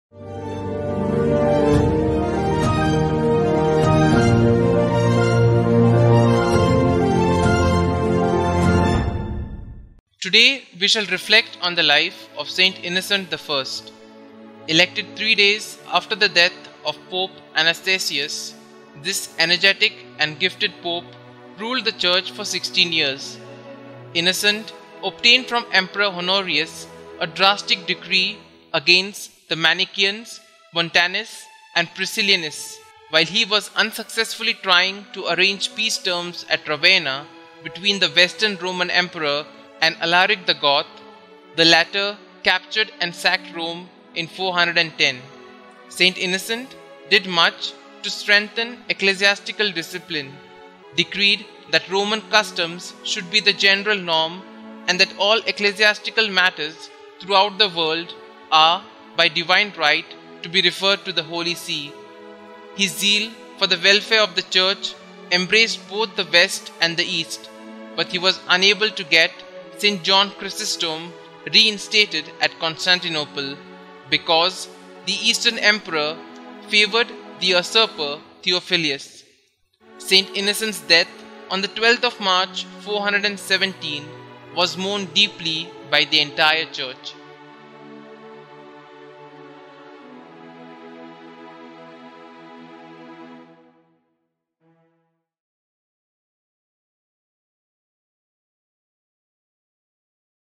St Innocent I - Saint of the Day with Fr Lindsay - 27 July 2024
Here is a brief reflection on the life of St Innocent I presented by Fr Lindsay Lobo